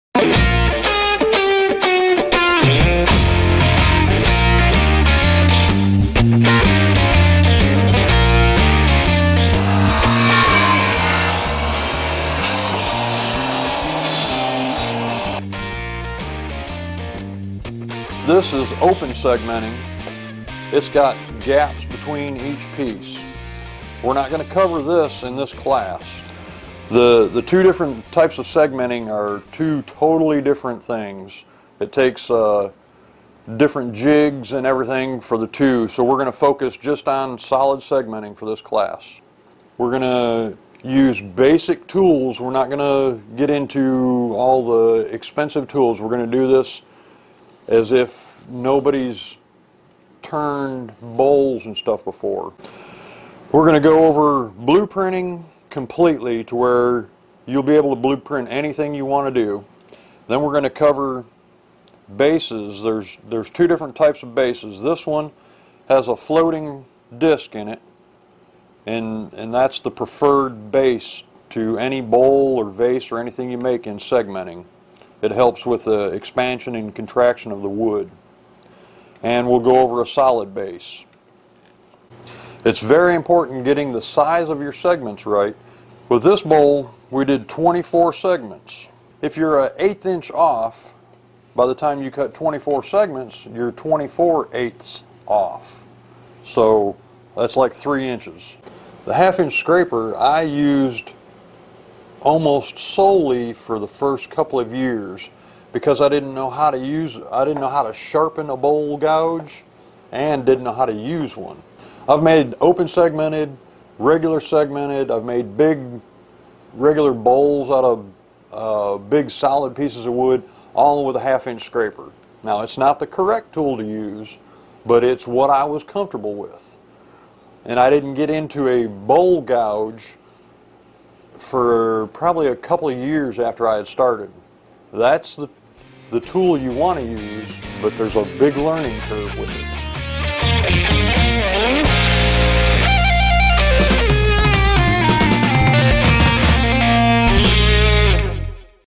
Segmenting online workshop with Bob Rundquist | TeachinArt
Introduction to segmenting is a 6 weeks online workshop with 4 extra weeks for reviewing. Bob is available during the class to answer all questions and will guide and support the online woodworkers.

Learn how to make a blueprint that can be altered for any other segmented bowl. Learn the right way to measure, different cutting techniques, gluing, sanding. Test different bottom techniques and learn basic turning techniques on the lathe.

This class is for closed segmenting. Here is the link to the contents and registration page on the TeachinArt website.